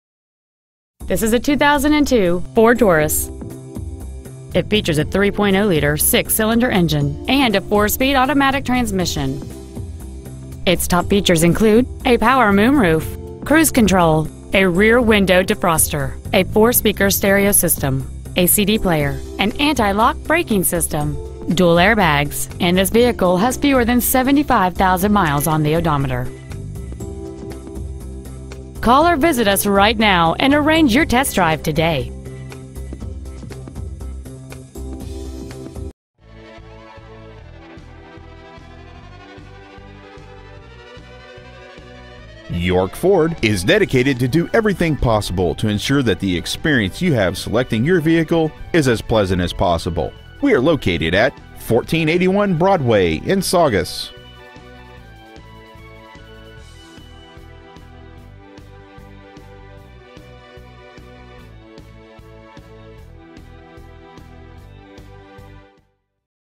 Preowned 2002 FORD TAURUS Saugus MA
Year: 2002
 Make: FORD
 Model: TAURUS
 Engine: 3.0L V6
 Trans.: AUTO 4SPD
 Exterior: DARK GRAY
 Miles: 74,210
 Interior: MEDIUM GRAPHITE